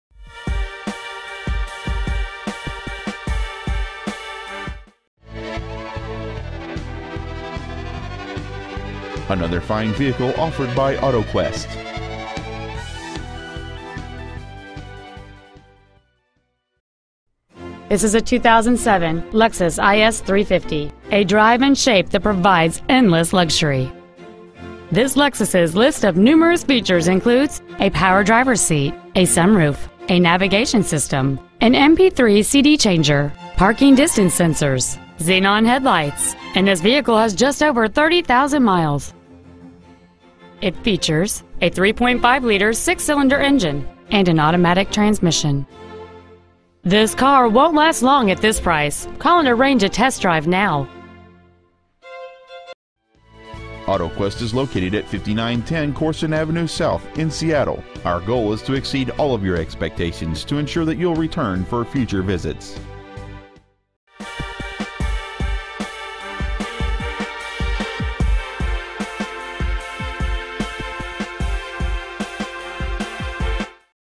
Preowned 2007 Lexus IS350 Seattle WA
We are proud to present this 2007 Lexus IS350.

 Year: 2007
 Make: Lexus
 Model: IS350
 Engine: 3.5L V6
 Trans.: Automatic
 Exterior: Black
 Miles: 30,154
 Interior: Black

 5910 Corson Ave S.

 Pre-Owned 2007 Lexus IS350 Seattle WA